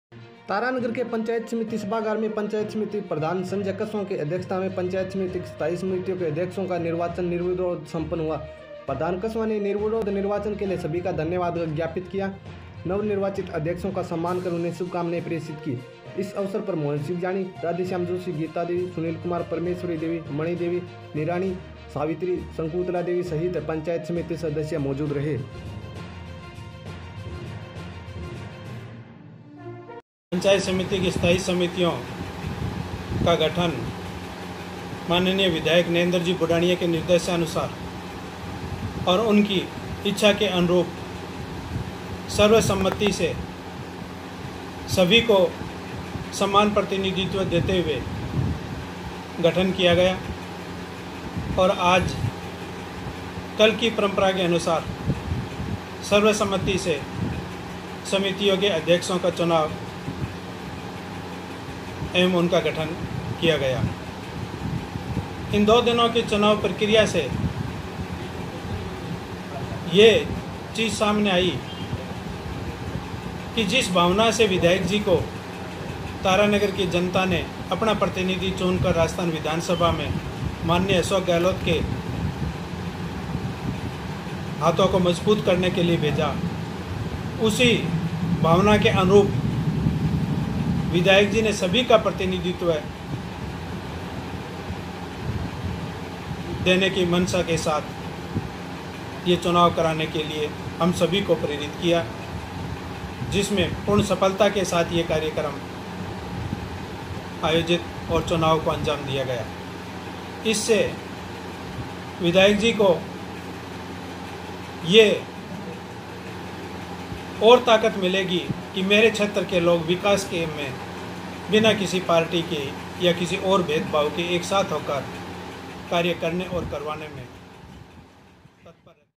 Tarangar: पंचायत समिति की स्थायी समितियों के अध्यक्षों का हुआ निर्विरोध निर्वाचन || JHN
तारानगर के पंचायत समिति सभागार में पंचायत समिति प्रधान संजय कस्वां की अध्यक्षता में पंचायत समिति की स्थायी समितियों के अध्यक्षों का निर्वाचन निर्विरोध सम्पन्न हुआ। प्रधान कस्वां ने निर्विरोध निर्वाचन के लिए सभी का धन्यवाद ज्ञापित किया। नवनिर्वाचित अध्यक्षों का सम्मान कर उन्हें शुभकामनाएं प्रेषित की। इस अवसर पर मोहरसिंह ज्याणी, राधेश्याम जोशी, गीता देवी, सुनील कुमार, परमेश्वरी देवी, मणी देवी, निराणी, सावित्री, शंकुतला देवी सहित सभी पंचायत समिति सदस्य मौजूद रहे।